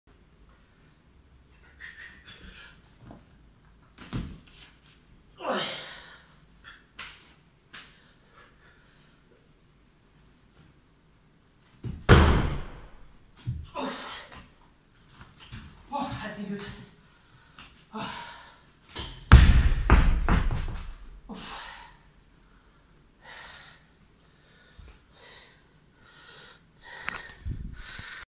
Atlas Stone. Strongman
First time lifting atlas Stone. This is 135kg and i did almost take the 160kg stone